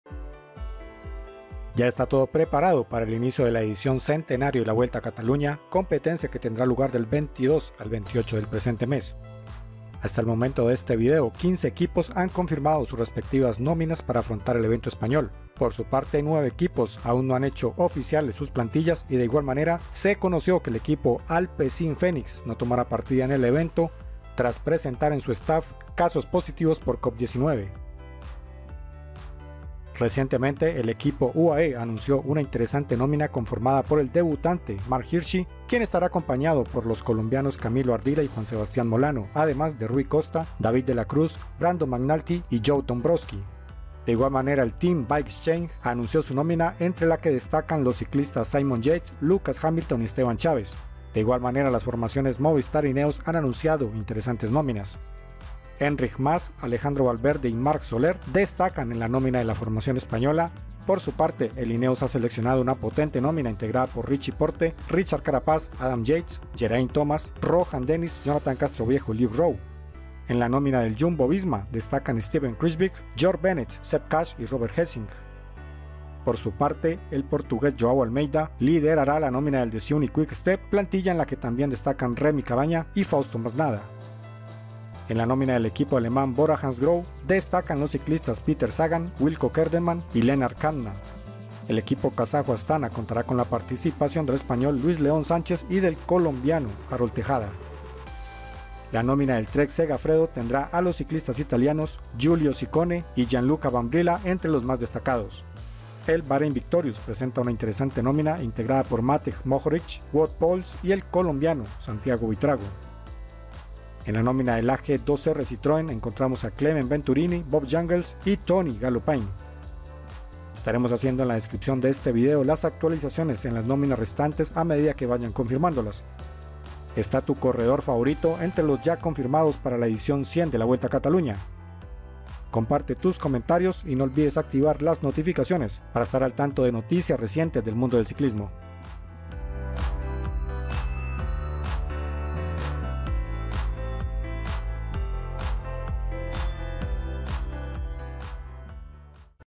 Atención! Noticias VUELTA a CATALUÑA 😮 ¡Baja Importante!
Atención! Equipo no participará!.  Ya está todo preparado para el inicio de la edición centenario de la Vuelta a Cataluña, edición que tendrá lugar del 22 al 28 de marzo.

🇪🇸 MOVISTAR TEAM ✅
– 🇪🇸 VALVERDE Alejandro
– 🇪🇸 MAS Enric
– 🇮🇹 CATALDO Dario
– 🇪🇸 VERONA Carlos
– 🇪🇸 SAMITIER Sergio
– 🇪🇸 PEDRERO Antonio
– 🇪🇸 SOLER Marc

🇳🇱 JUMBO-VISMA ✅
– 🇳🇿 BENNETT George
– 🇺🇸 KUSS Sepp
– 🇳🇱 KRUIJSWIJK Steven
– 🇳🇱 GESINK Robert
– 🇳🇱 BOUWMAN Koen
– 🇳🇱 TOLHOEK Antwan
– 🇦🇺 HARPER Chris

🇧🇪 DECEUNINCK-QUICK STEP ✅
– 🇵🇹 ALMEIDA João
– 🇮🇹 MASNADA Fausto
– 🇨🇿 ČERNÝ Josef
– 🇬🇧 KNOX James
– 🇧🇪 DEVENYNS Dries
– 🇧🇪 SERRY Pieter
– 🇫🇷 CAVAGNA Rémi

🇦🇪 UAE TEAM EMIRATES ✅
– 🇵🇹 COSTA Rui
– 🇺🇸 MCNULTY Brandon
– 🇨🇭 HIRSCHI Marc
– 🇨🇴 MOLANO Juan Sebastián
– 🇺🇸 DOMBROWSKI Joe
– 🇨🇴 ARDILA Andrés Camilo
– 🇪🇸 DE LA CRUZ David

🇬🇧 INEOS GRENADIERS ✅
– 🇦🇺 PORTE Richie
– 🇪🇨 CARAPAZ Richard
– 🇬🇧 YATES Adam
– 🇬🇧 THOMAS Geraint
– 🇦🇺 DENNIS Rohan
– 🇪🇸 CASTROVIEJO Jonathan
– 🇬🇧 ROWE Luke

🇩🇪 TEAM DSM ✅
– 🇦🇺 HINDLEY Jai
– 🇦🇺 STORER Michael
– 🇺🇸 HAGA Chad
– 🇦🇺 HAMILTON Chris
– 🇩🇪 KANTER Max
– 🇮🇪 ROCHE Nicolas
– 🇳🇱 ARENSMAN Thymen

🇩🇪 BORA-HANSGROHE ✅
– 🇸🇰 SAGAN Peter
– 🇳🇱 KELDERMAN Wilco
– 🇩🇪 KÄMNA Lennard
– 🇧🇪 MEEUS Jordi
– 🇳🇱 SCHELLING Ide
– 🇩🇪 ZWIEHOFF Ben
– 🇩🇰 WANDAHL Frederik

🇰🇿 ASTANA-PREMIER TECH ✅
– 🇪🇸 RODRÍGUEZ Óscar
– 🇪🇸 SÁNCHEZ Luis León
– 🇰🇿 PRONSKIY Vadim
– 🇩🇰 GREGAARD Jonas
– 🇨🇴 TEJADA Harold
– 🇿🇦 DE BOD Stefan
– 🇪🇷 KUDUS Merhawi

🇺🇸 TREK-SEGAFREDO ✅
– 🇮🇹 BRAMBILLA Gianluca
– 🇫🇷 ELISSONDE Kenny
– 🇪🇸 LÓPEZ Juan Pedro
– 🇮🇹 CICCONE Giulio
– 🇱🇺 RIES Michel
– 🇩🇰 KAMP Alexander
– 🇩🇰 SKJELMOSE JENSEN Mattias

🇦🇺 TEAM BIKEEXCHANGE ✅
– 🇬🇧 YATES Simon
– 🇨🇴 CHAVES Esteban
– 🇪🇪 KANGERT Tanel
– 🇦🇺 HAMILTON Lucas
– 🇳🇿 SMITH Dion
– 🇺🇸 BOOKWALTER Brent
– 🇦🇺 SCOTSON Callum

🇧🇭 BAHRAIN-VICTORIOUS ✅
– 🇳🇱 POELS Wout
– 🇦🇹 PERNSTEINER Hermann
– 🇨🇴 BUITRAGO Santiago
– 🇸🇮 MOHORIČ Matej
– 🇳🇱 INKELAAR Kevin
– 🇸🇮 NOVAK Domen
– 🇬🇧 WILLIAMS Stephen

🇫🇷 AG2R CITROËN ✅
– 🇫🇷 VENTURINI Clément
– 🇫🇷 CHAMPOUSSIN Clément
– 🇱🇺 JUNGELS Bob
– 🇫🇮 HÄNNINEN Jaakko
– 🇱🇺 GASTAUER Ben
– 🇫🇷 GALLOPIN Tony
– 🇫🇷 BIDARD François

🇺🇸 RALLY CYCLING ✅
– 🇺🇸 ROSSKOPF Joey
– 🇺🇸 MANNION Gavin
– 🇺🇸 JOYCE Colin
– 🇺🇸 SWIRBUL Keegan
– 🇨🇦 ZUKOWSKY Nickolas
– 🇺🇸 BROWN Nathan
– 🇨🇦 BRITTON Rob

🇪🇸 EQUIPO KERN PHARMA ✅
– 🇪🇸 MARQUEZ Marti
– 🇪🇸 CASTRILLO Jaime
– 🇪🇸 GALVÁN Francisco
– 🇪🇸 BERRADE Urko
– 🇷🇺 NOVIKOV Savva
– 🇨🇴 MENDEZ Daniel Alejandro
– 🇪🇸 ADRIÀ Roger

🇪🇸 EUSKALTEL-EUSKADI ✅
– 🇪🇸 MATÉ Luis Ángel
– 🇪🇸 SOTO Antonio Jesus
– 🇪🇸 BIZKARRA Mikel
– 🇪🇸 LOBATO Juan José
– 🇪🇸 BOU Joan
– 🇪🇸 ITURRIA Mikel
– 🇪🇸 MARTÍN Gotzon

🇫🇷 TEAM ARKÉA SAMSIC ✅
– 🇨🇴 QUINTANA Nairo
– 🇫🇷 DELAPLACE Anthony
– 🇵🇱 OWSIAN Łukasz
– 🇫🇷 LOUVEL Matis
– 🇫🇷 PICHON Laurent
– 🇫🇷 GESBERT Élie
– 🇫🇷 RIOU Alan

🇮🇱 ISRAEL START-UP NATION ✅
– 🇬🇧 FROOME Chris
– 🇮🇪 MARTIN Dan
– 🇨🇦 WOODS Michael
– 🇿🇦 IMPEY Daryl
– 🇨🇦 CATAFORD Alexander
– 🇮🇱 GOLDSTEIN Omer
– 🇨🇭 HOLLENSTEIN Reto